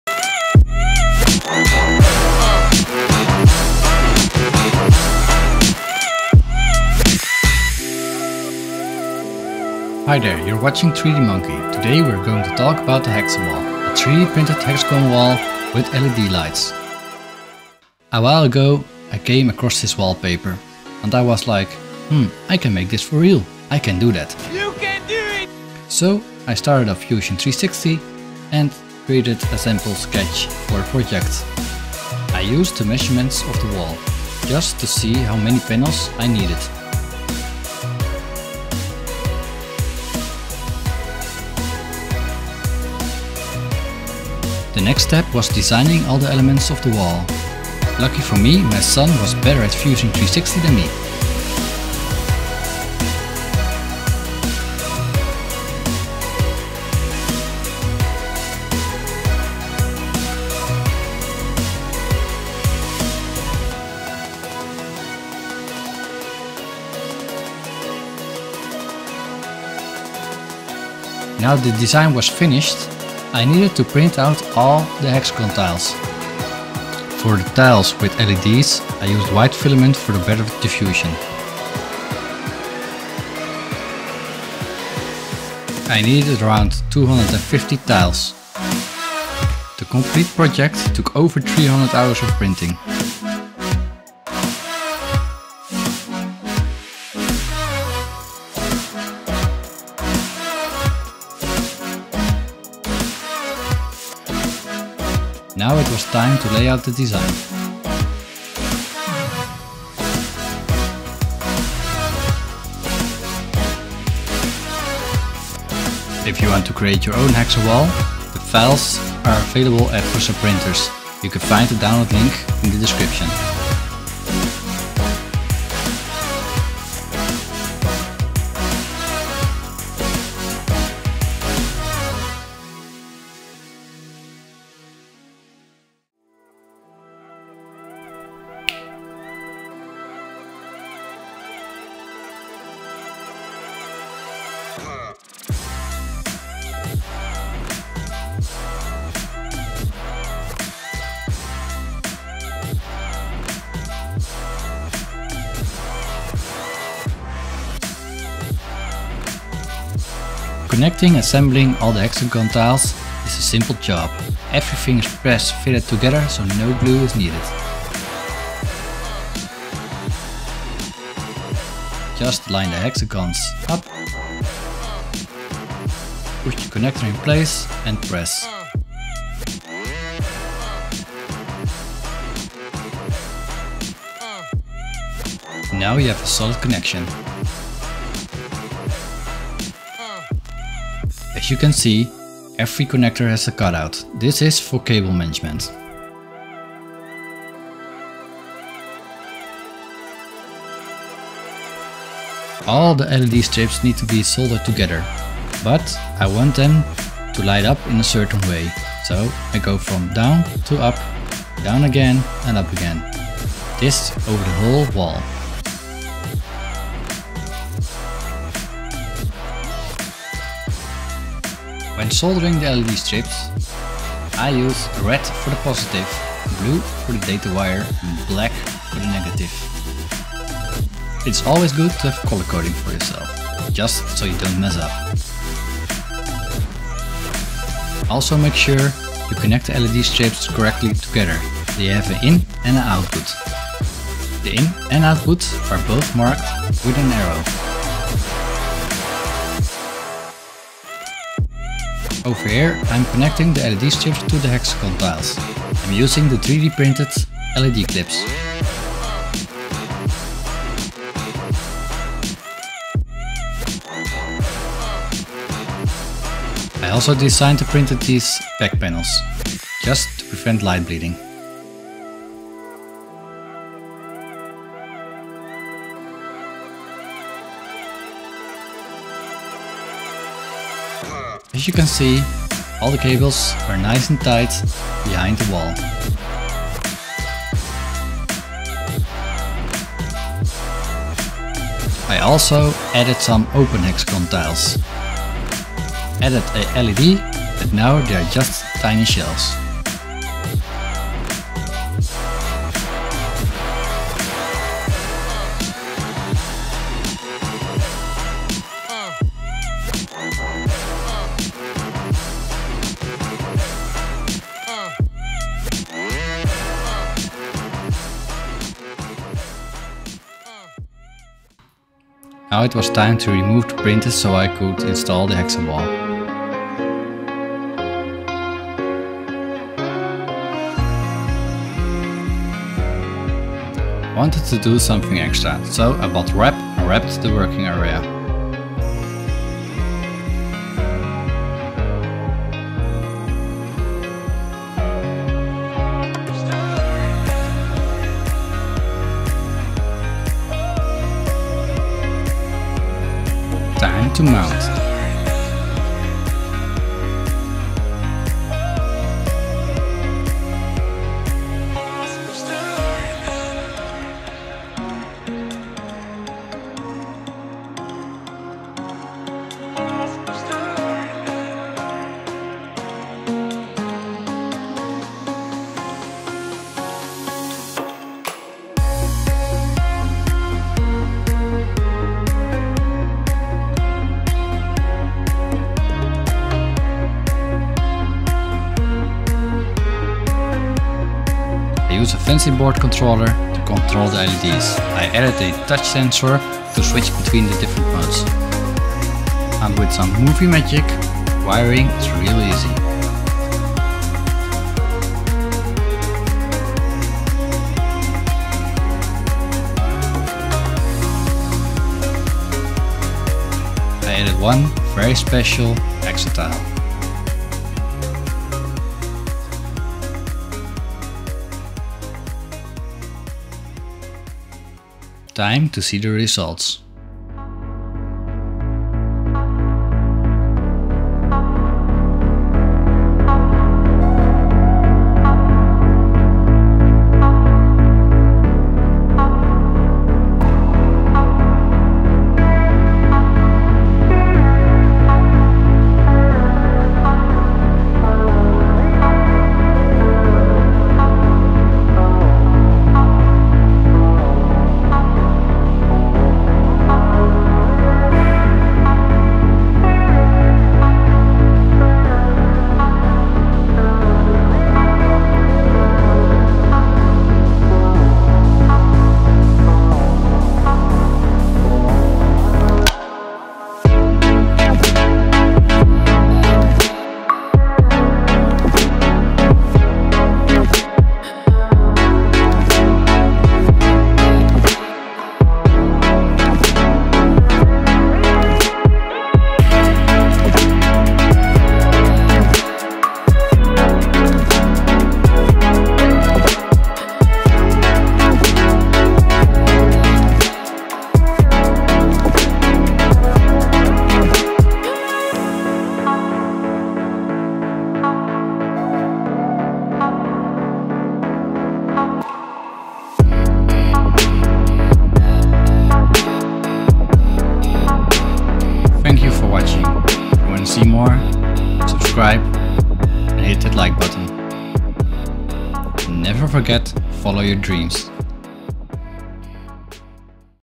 3D printed another wall my 2 year old designed and it lights up!
The Hexawall is a complete 3D printed wall, it has led strips and gives a real luxury feeling at night. And the best, you can download the files for free so you can build your own!

Some of the hardware i use:
- The controller i use is the fancyboard.io controller.
- Led strips are digital WS2812B 5V led strips.
- I use a 20watt 5V power supply.